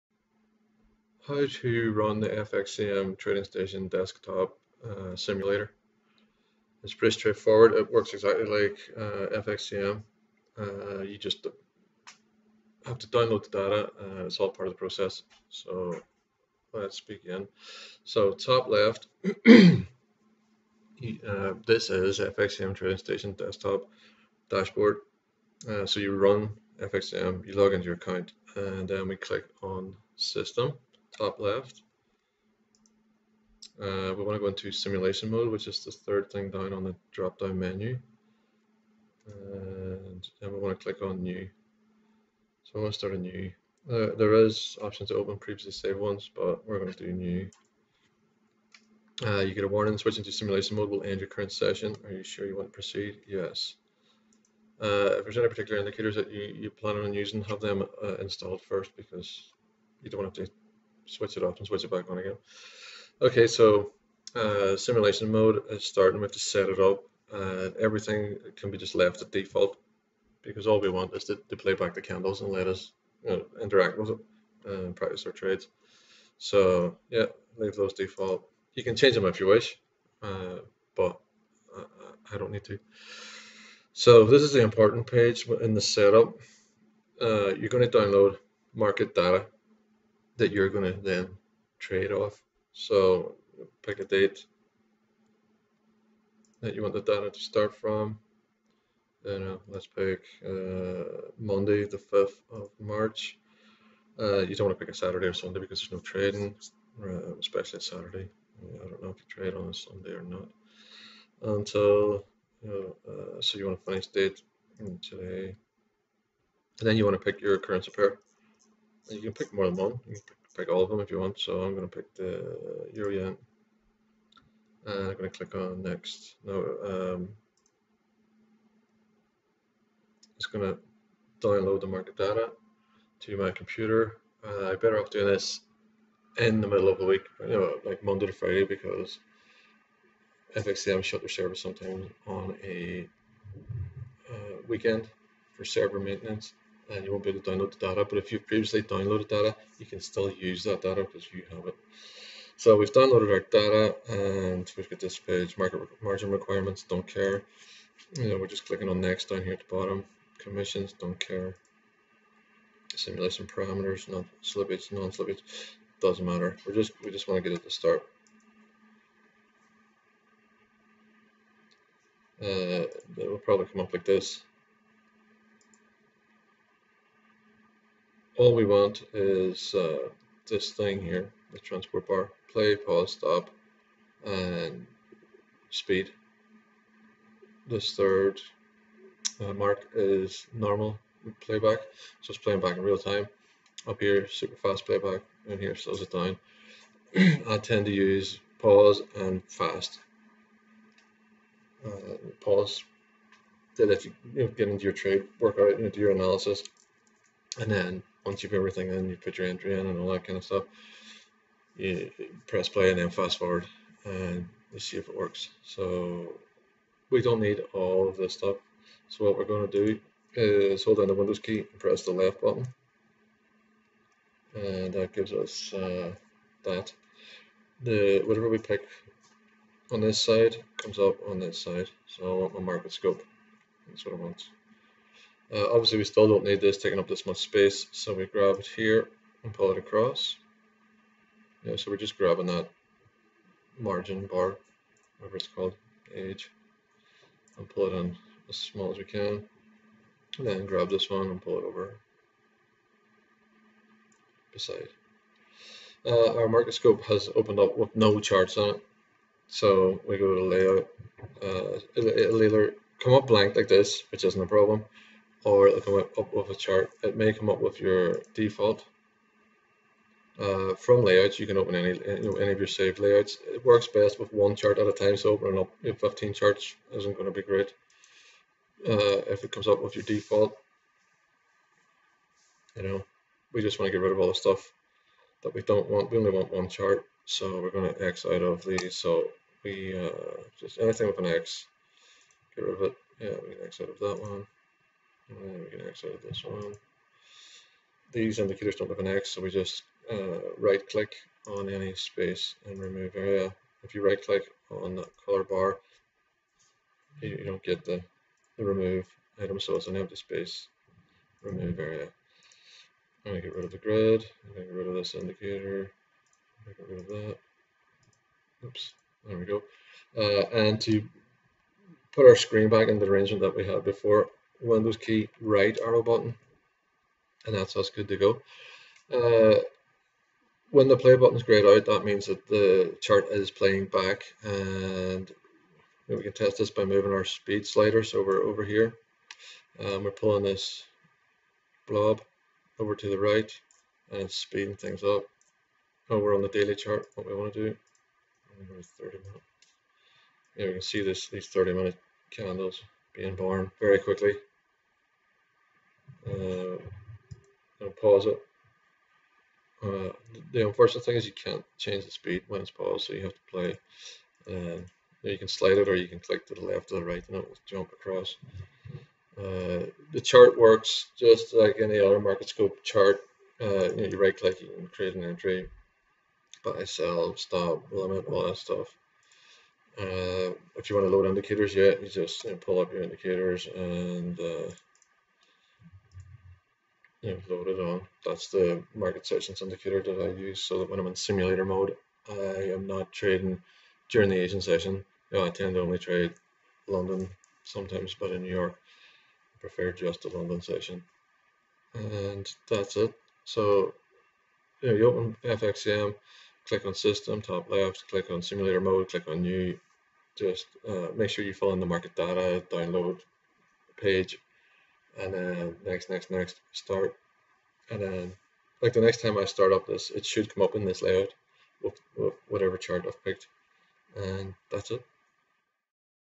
FXCM Simulator Setup
A quick video on how to set up the simulator in FXCM trade station